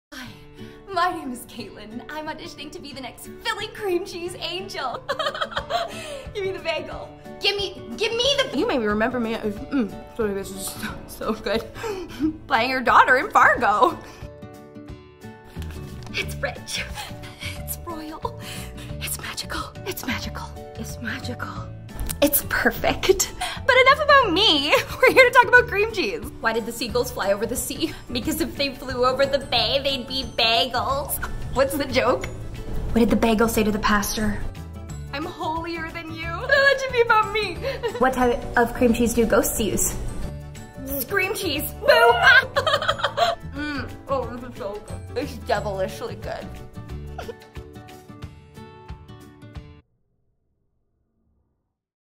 CREAM CHEESE COMMERCIAL!!
I had the best time creating and editing this video for PHILLYcastingcall - It was such a blast, and my boyfriend and I apparently found our niche - CREAM CHEESE. Thanks for the opportunity!